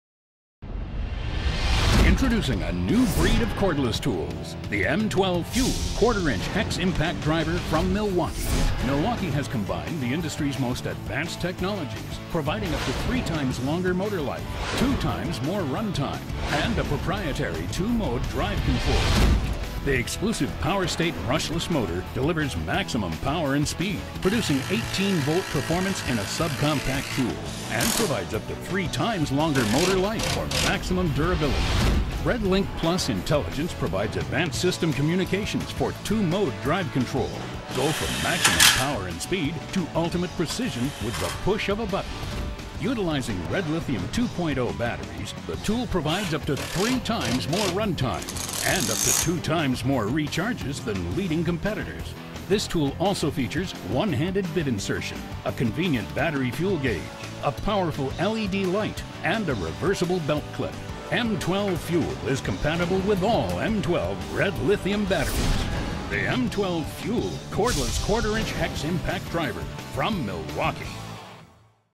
Ace Tool | Milwaukee Tool M12 Fuel Impact Driver 2597-22
The M12 FUEL™ 1/4" Hex Impact Driver delivers 18V fastening performance, in an extremely compact, lightweight design.